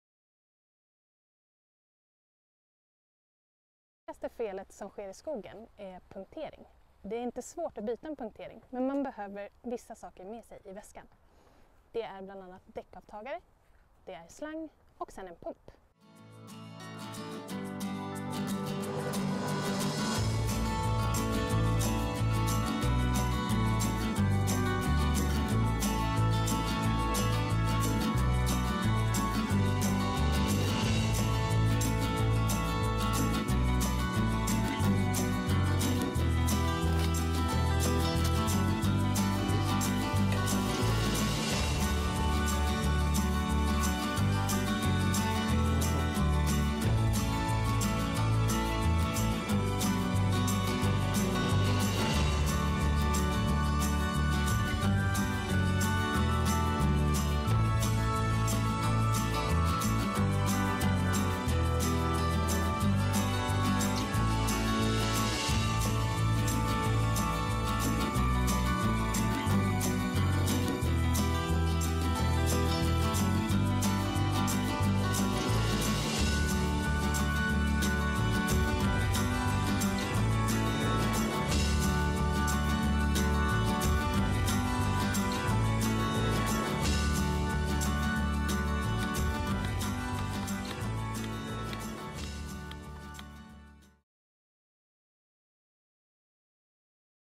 Laga punka på cykeln
Ibland händer det, en punka är inte hela världen bara prylarna och kunskapen finns,